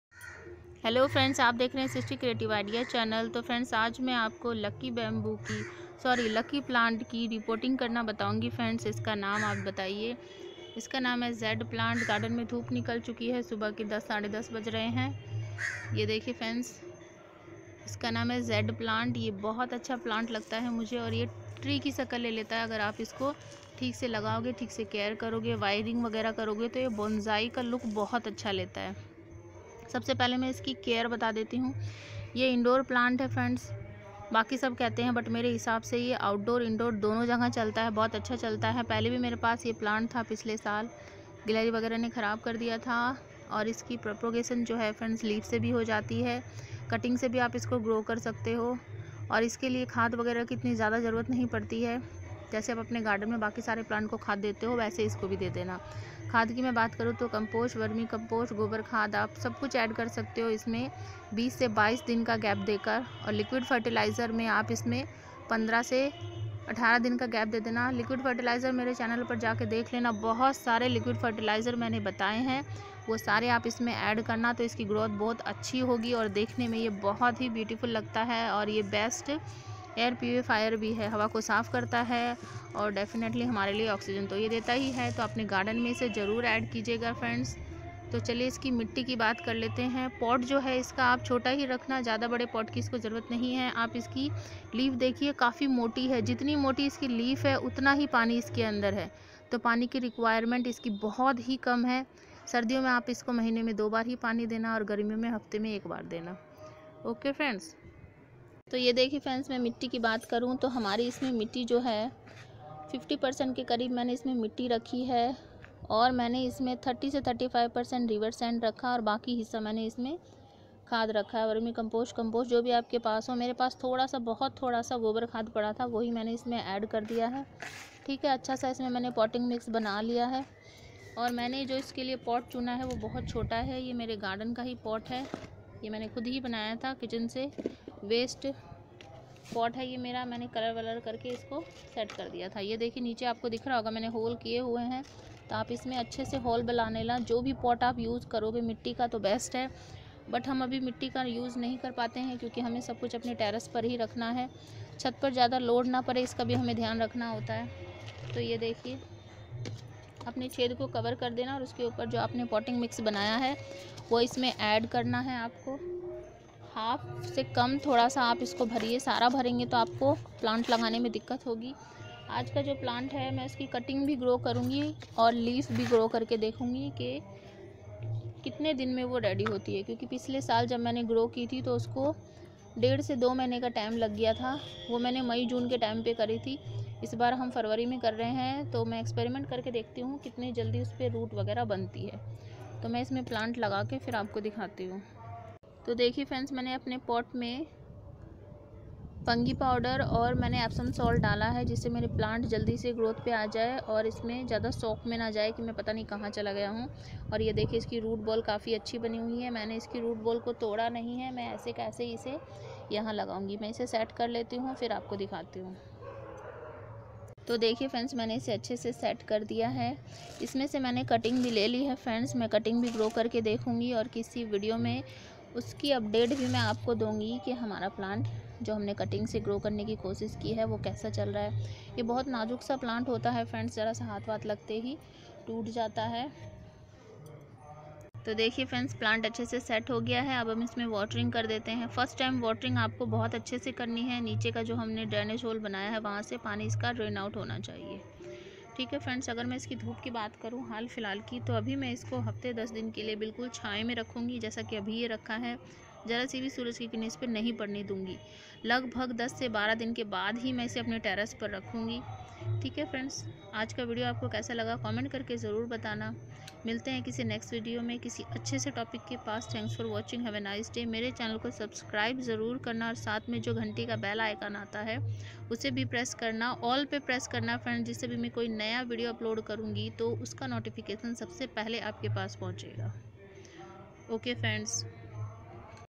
Lucky (Z) plant ki care and tips.
Hello friends aj main apko lucky plant ki reporting and care btaugi,
My privous video